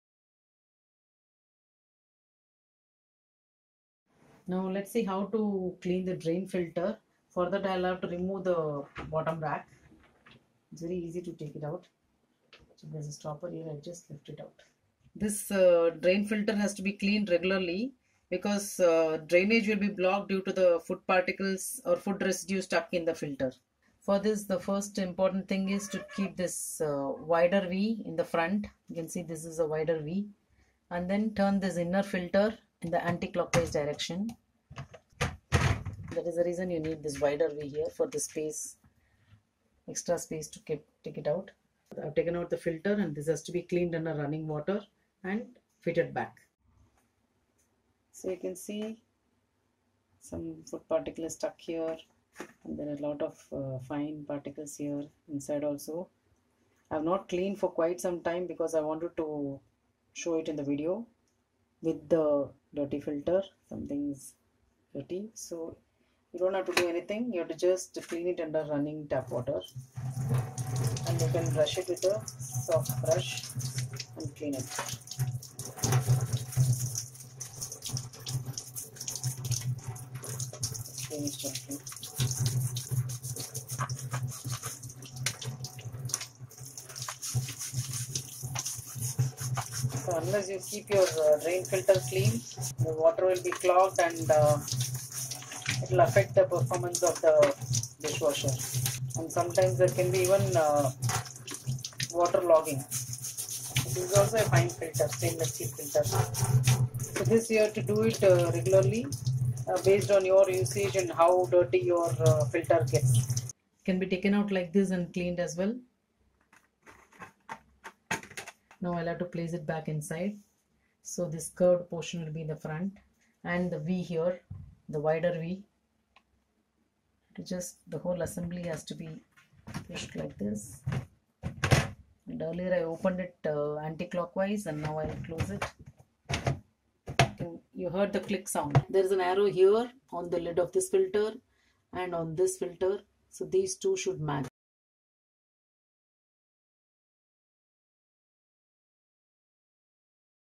How to clean drain filter in LG Dishwasher DFB424FP | Part VI
To buy this dishwasher -
In this video I have shown a very important maintenance aspect of dishwasher which is the drain filter cleaning.
You will need to clean the drain filter on a regular basis depending on the level of dirt accumulation.